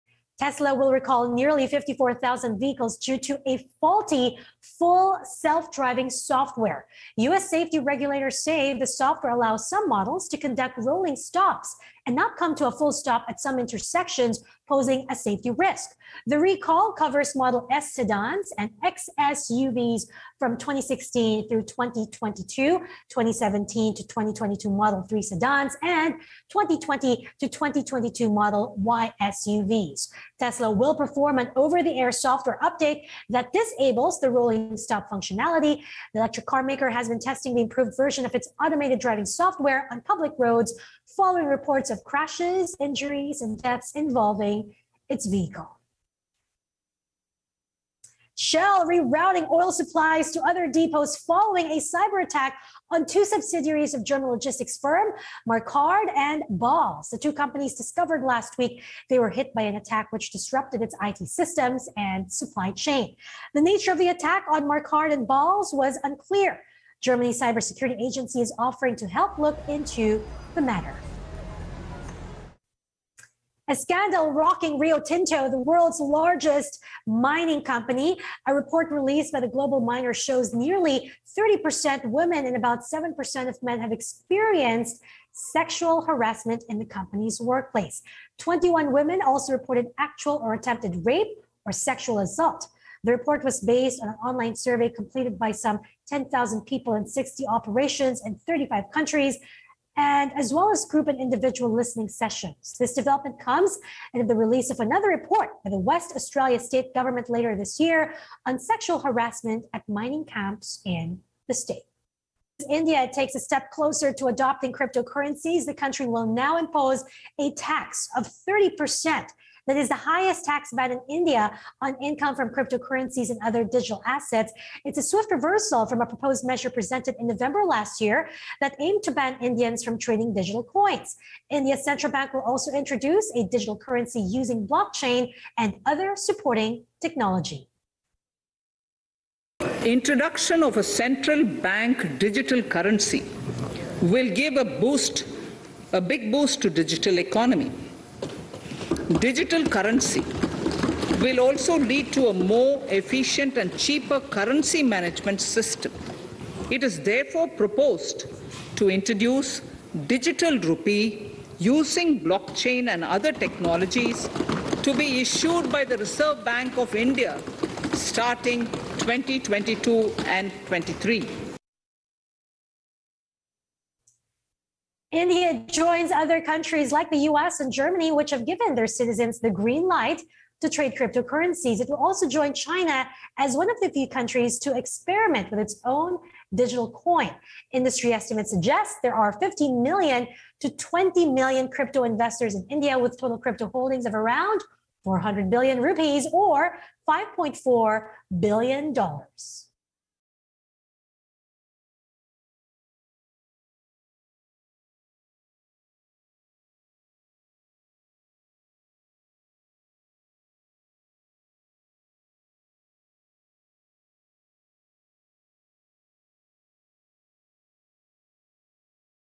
Tesla to recall nearly 54,000 vehicles with faulty software | ANC
Market Edge: Tesla will recall nearly 54,000 vehicles due to a faulty full self-driving software.